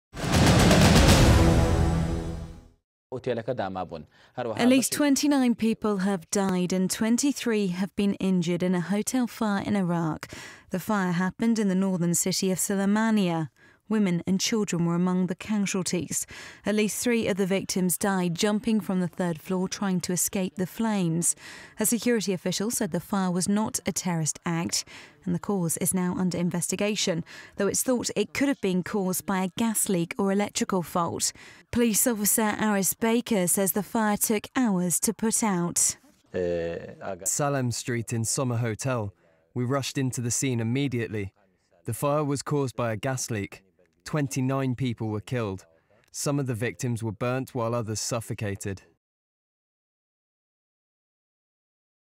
At least thirty killed in fire in Iraqi hotel fire
At least thirty people have been killed in a hotel fire in the Iraqi city of Sulaimaniya. .